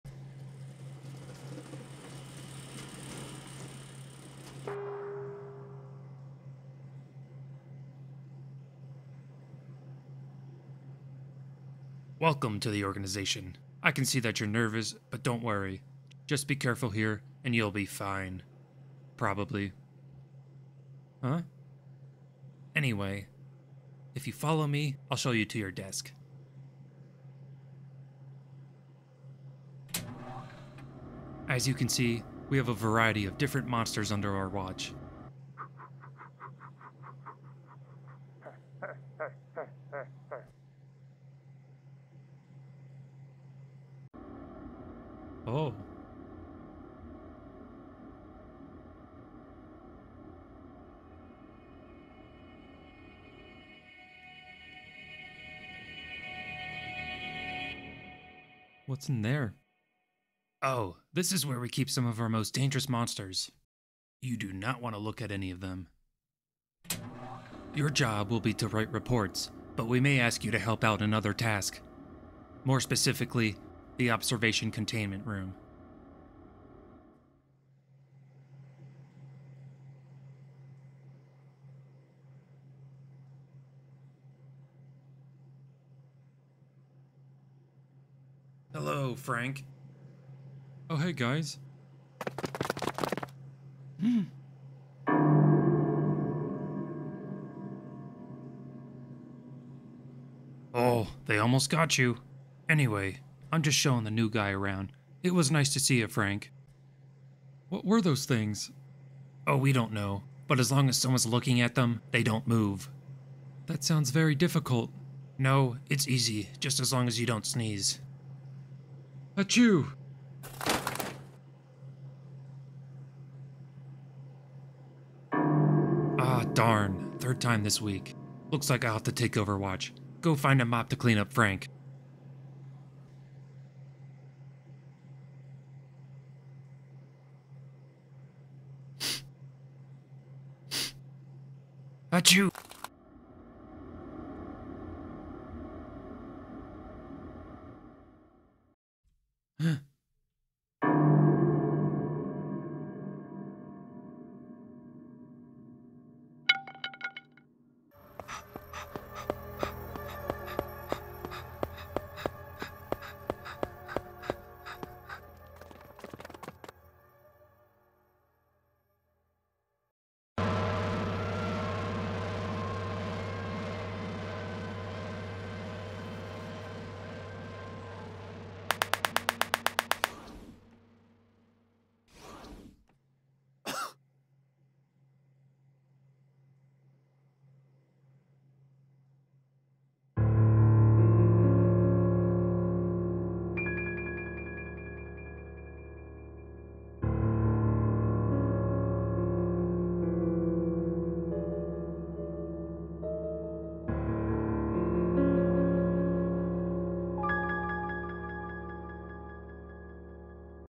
Monster Watchers
In this parody animation, the Monster Watchers are a less successful version of a well known entity containment organization. The Monster Watchers maintain little standards and can barely keep themselves staffed.